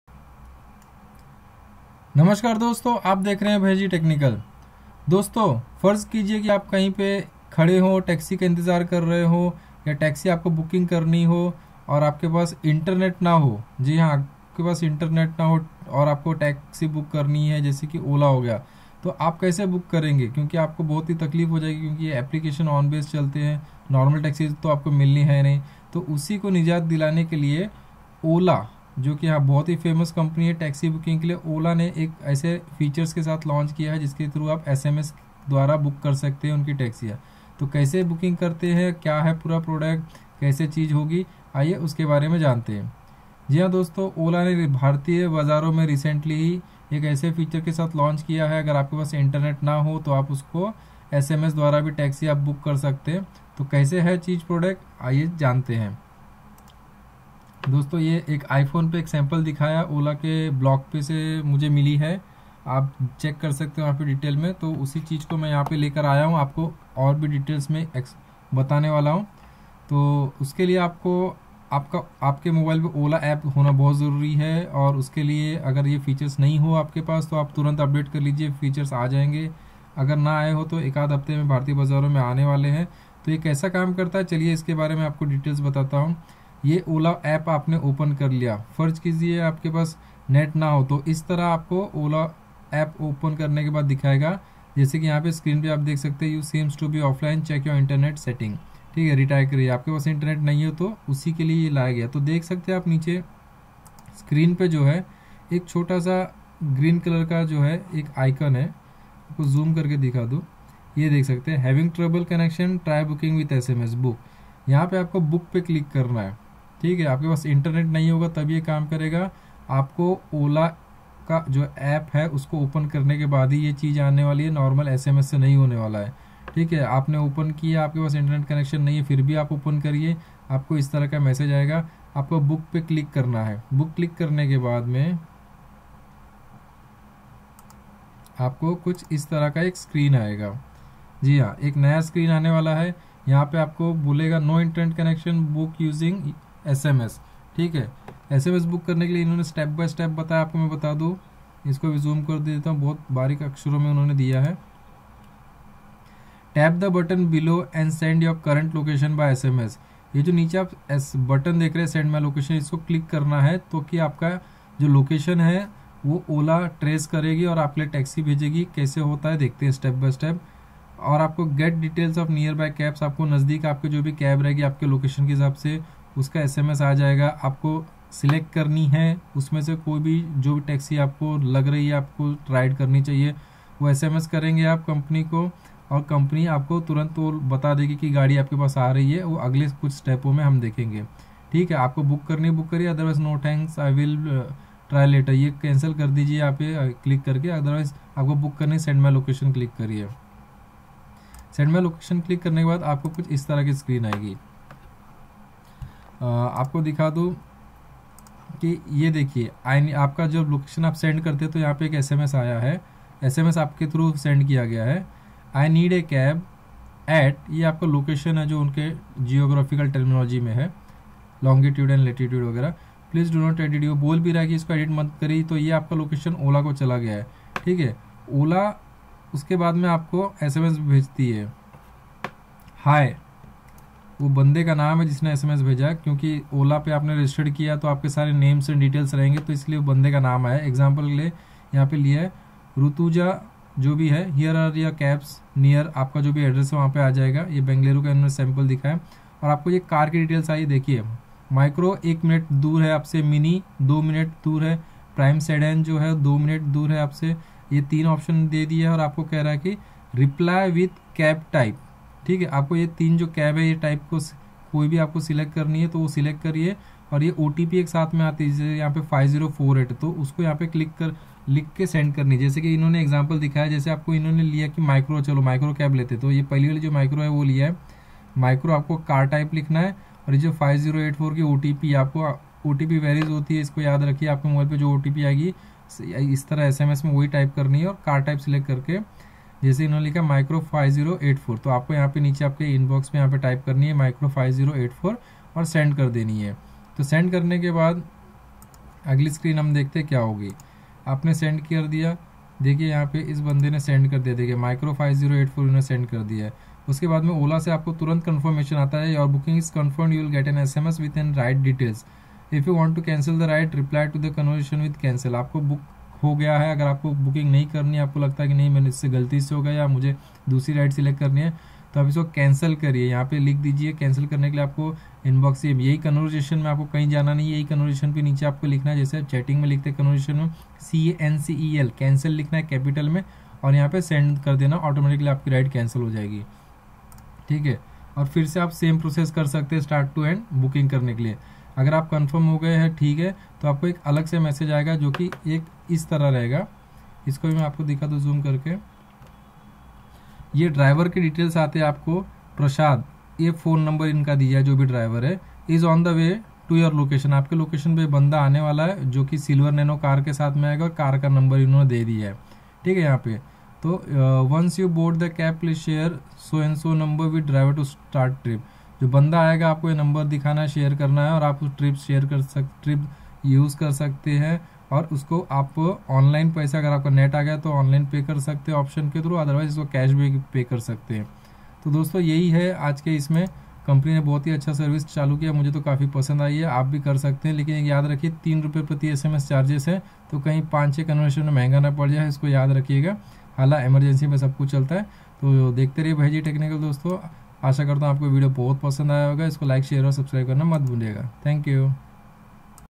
OLA booking on SMS
OLA a Car booking App has came up with a feature through which a user can book a ride through SMS in case failure of Internet.